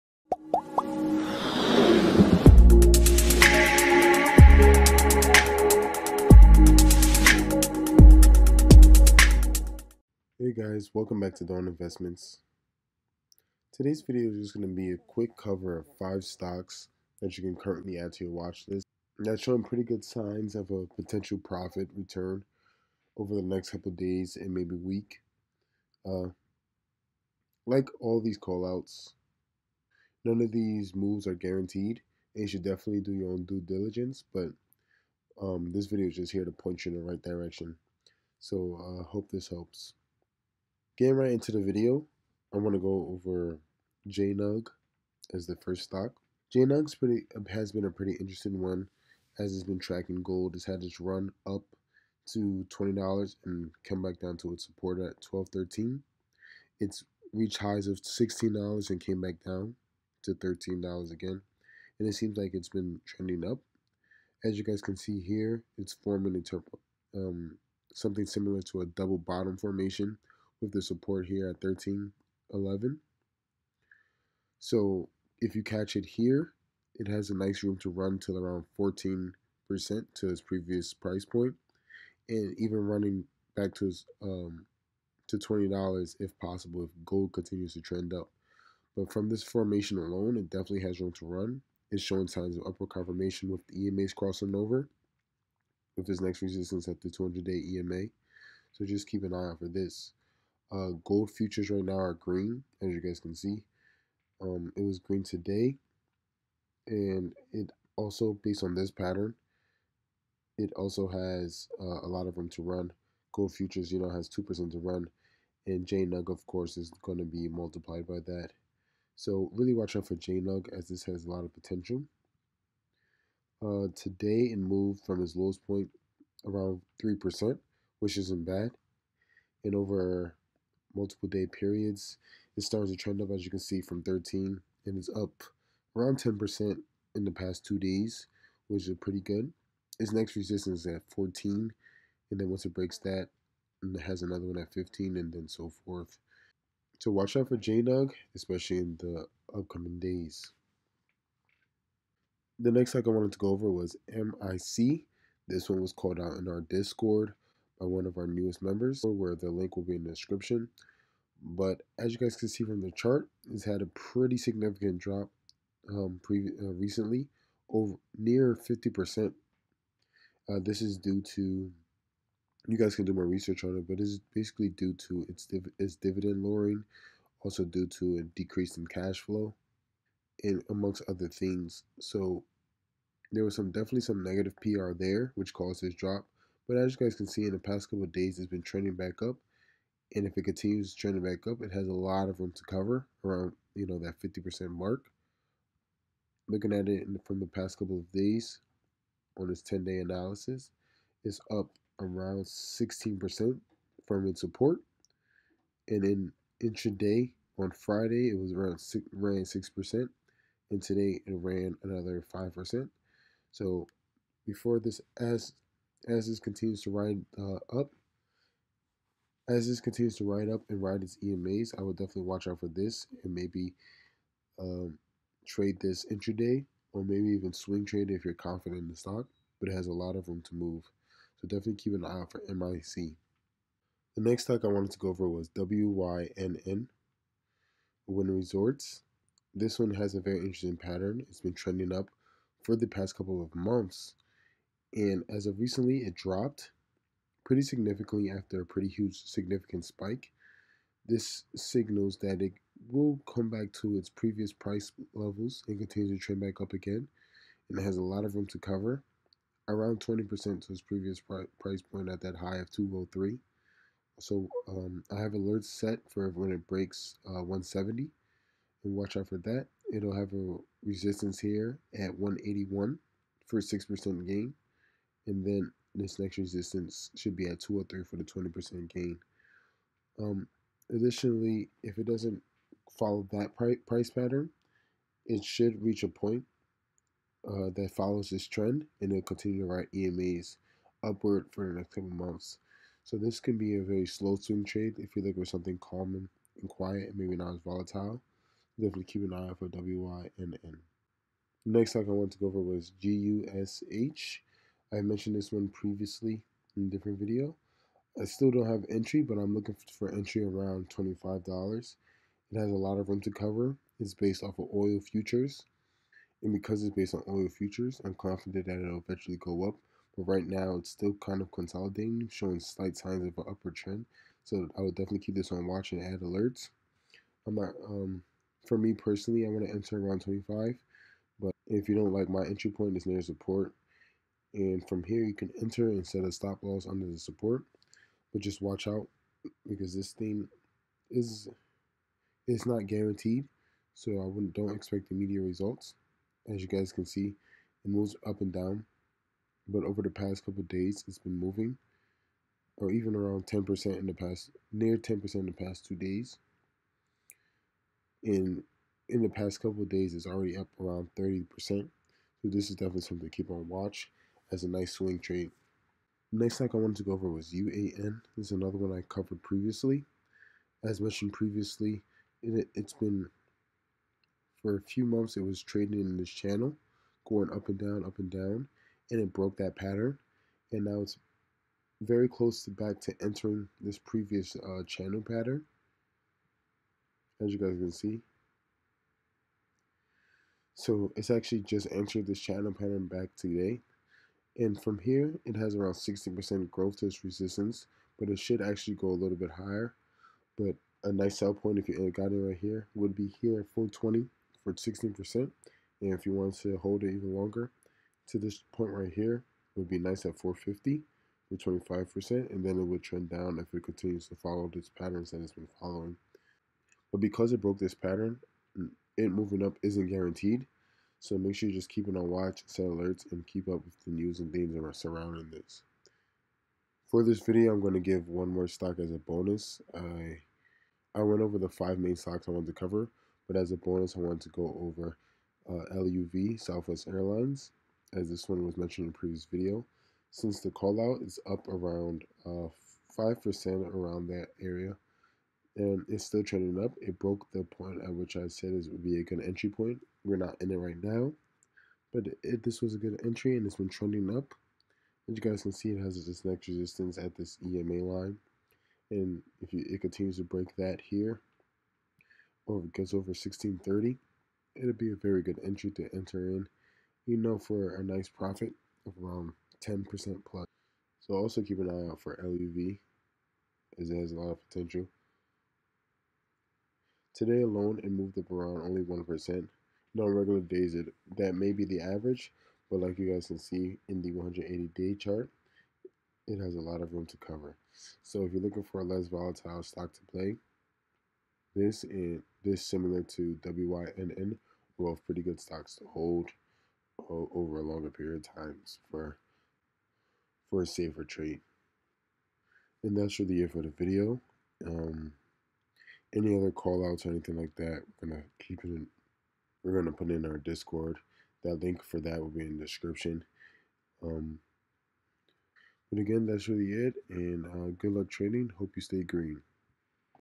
Top 5 Stocks for the last week of February 2018 | Monday Stock Review
Monday stock review. 5 stocks with 1 bonus extra that you can trade this week for great profits

Free GroupChat link Below

Join Our New Discord Group which is a free stock Group chat for more exclusive Stocks on a daily: